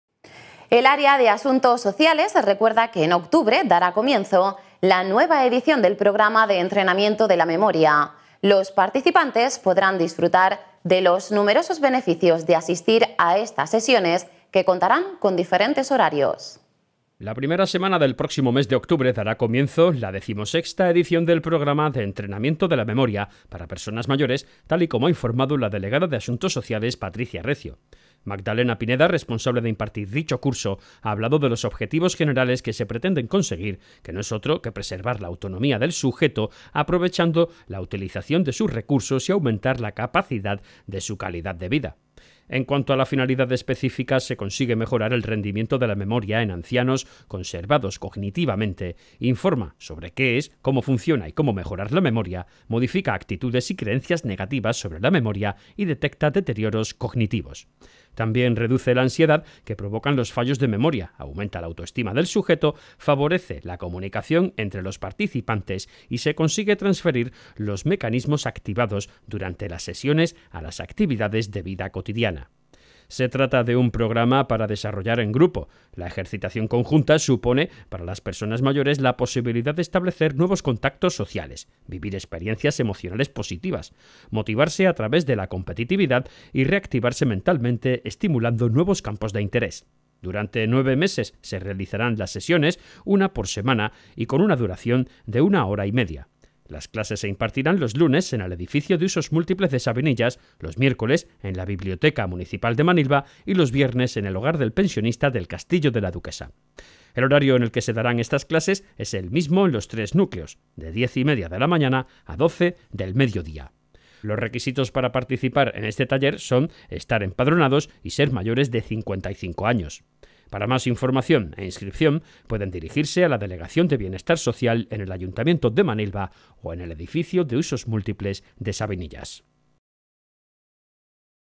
EN OCTUBRE COMENZARÁ LA XVI EDICIÓN DEL PROGRAMA DE ENTRENAMIENTO DE LA MEMORIA
La primera semana del próximo mes de octubre dará comienzo la XVI edición del programa de Entrenamiento de la Memoria para personas mayores, tal y como ha informado la delegada de Asuntos Sociales, Patricia Recio.

Magdalena Pineda, responsable de impartir dicho curso, ha hablado de los objetivos generales que se pretenden conseguir, que no es otro que preservar la autonomía del sujeto aprovechando la utilización de sus recursos y aumentar la capacidad de su calidad de vida.

En cuanto a la finalidad específica se consigue mejorar el rendimiento de la memoria en ancianos conservados cognitivamente; Informa sobre qué es, cómo funciona y cómo mejorar la memoria; Modifica actitudes y creencias negativas sobre la memoria y Detecta deterioros cognitivos.
También reduce  la ansiedad que provocan los fallos de memoria; aumenta la autoestima del sujeto; favorece la comunicación entre los participantes y se consigue transferir los mecanismos activados durante las sesiones a las actividades de vida cotidiana

Se trata de un programa para desarrollar en grupo. La ejercitación conjunta supone para las personas mayores la posibilidad de establecer nuevos contactos sociales, vivir experiencias emocionales positivas, motivarse a través de la competitividad y reactivarse mentalmente estimulando nuevos campos de intereses.
Durante nueve meses se realizarán las sesiones una vez por semana y con una duración de una hora y media.
Las clases se impartirán los lunes en el Edificio de Usos Múltiples de Sabinillas: los miércoles en la Biblioteca Municipal de Manilva y los viernes en el Hogar del Pensionista de El Castillo de la Duquesa.
El horario en el que se darán estas clases es el mismo en los tres núcleos, de diez y media de la mañana a doce del mediodía.
Los requisitos para participar en este taller será estar empadronados y ser mayores de 55 años.
Para más información e inscripción pueden dirigirse a la Delegación de Bienestar Social en el Ayuntamiento de Manilva o en el edificio de Usos Múltiples en Sabinillas.